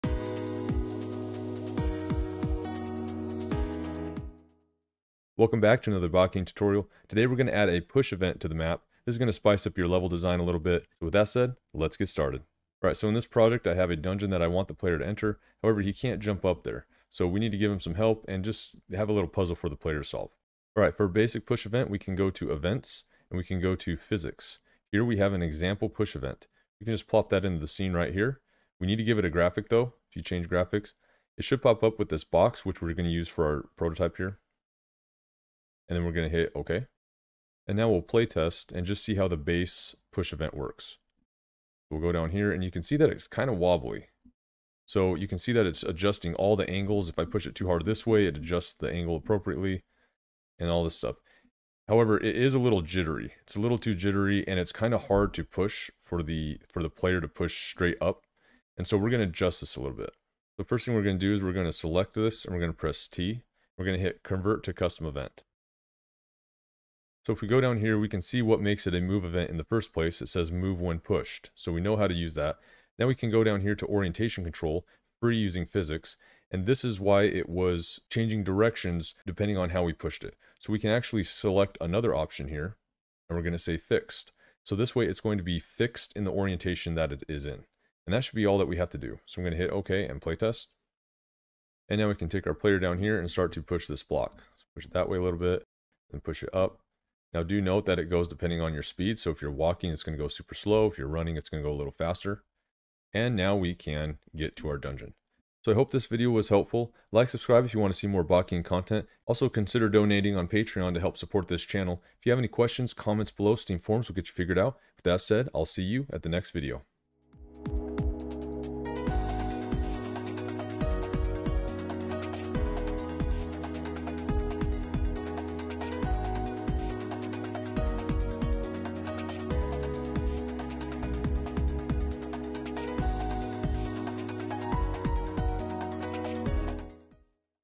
Pushable Objects - RPG Developer Bakin
RPG Developer Bakin tutorial how to create a pushable object.

Special Thanks (Patron Reward):
Benjamin Humphrey, myOmochi, and BlackWhite!